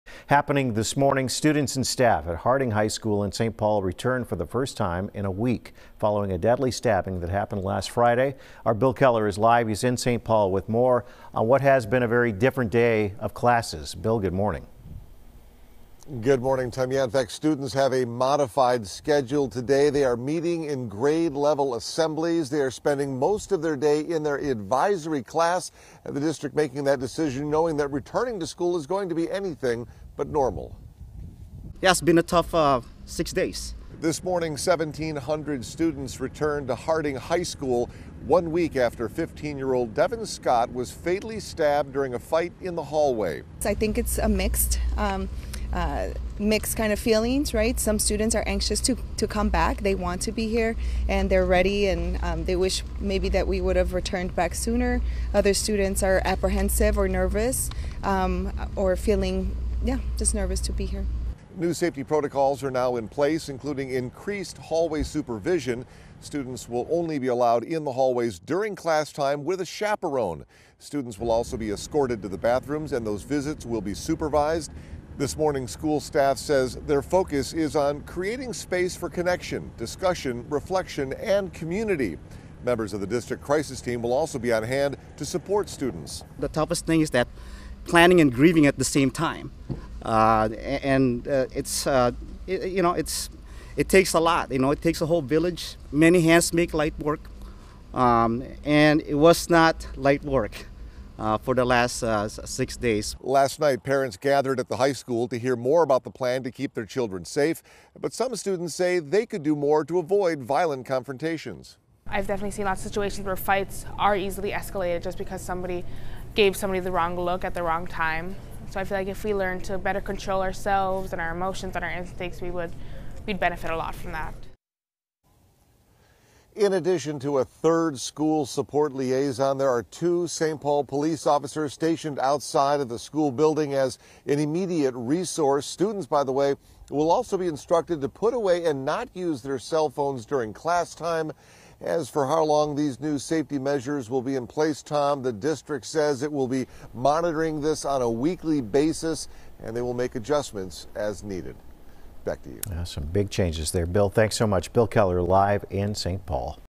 Harding High School students return week after deadly stabbing
Students at Harding High School returned to the building on Friday, a week after a fatal stabbing in the hallway. The district has instituted additional safety protocols, and students will be dismissed early.